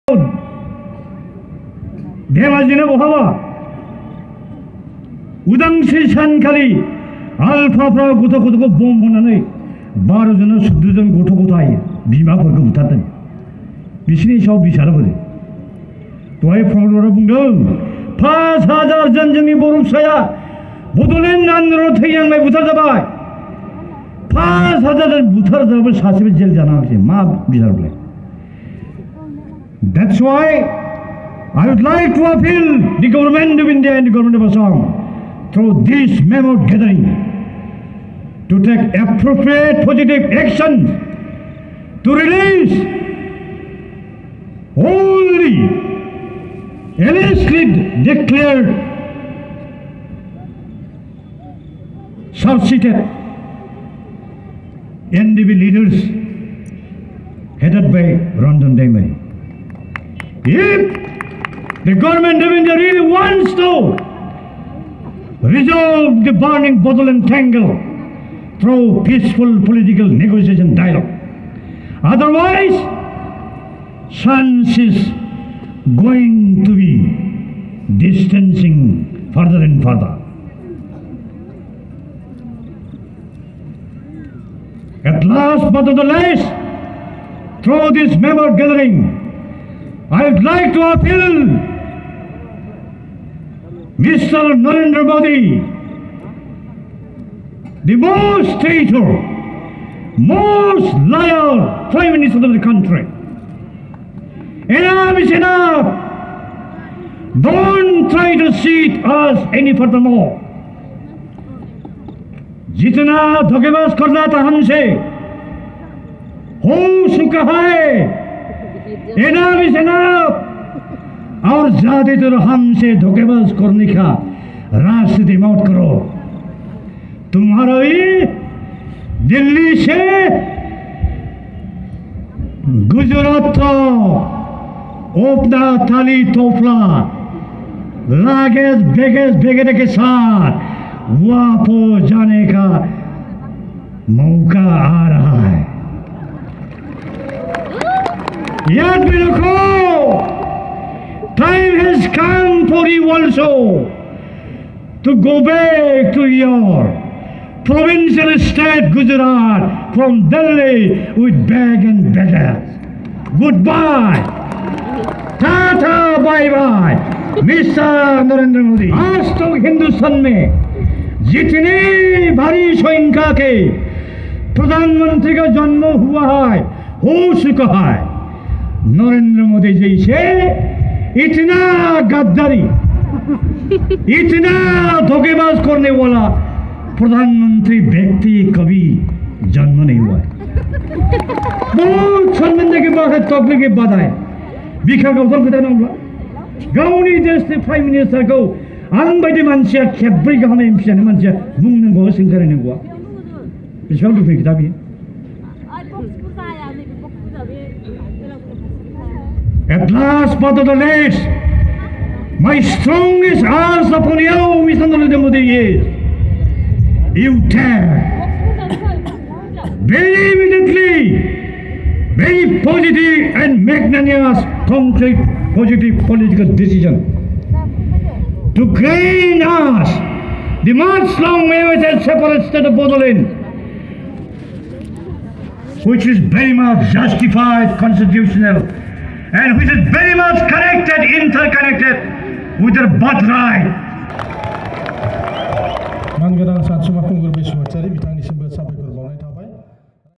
Sansuma Kungur Basumatary Assam Ex MP || Special Speech at Rowta Rally 2019
Super Hit Bodo Video:-